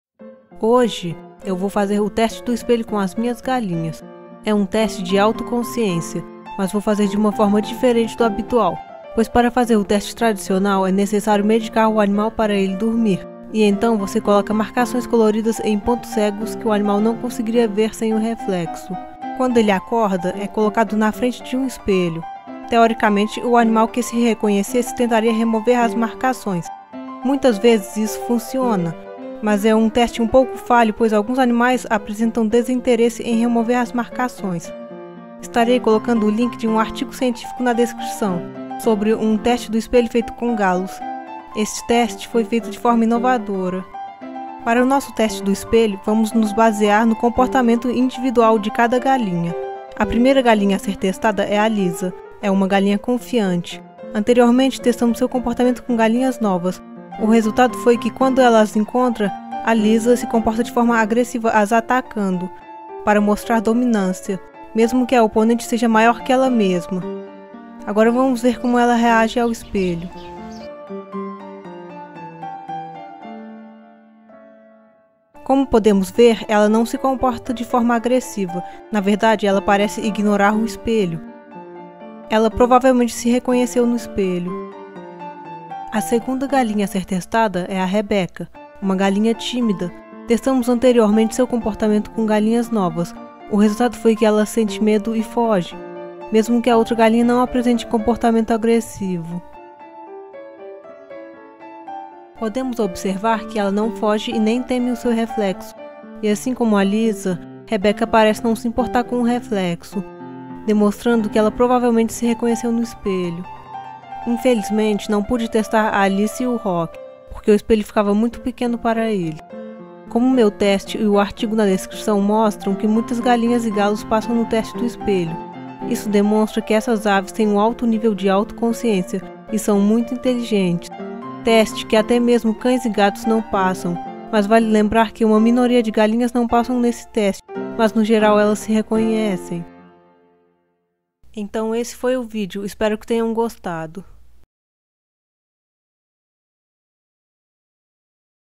Fiz o Teste do Espelho Com as Minhas Galinhas
Hoje vamos fazer o teste do espelho com as minhas galinhas, mas de uma forma diferente do habitual, vamos nos basear no comportamento individual de galinha para entender se elas se identificaram no espelho.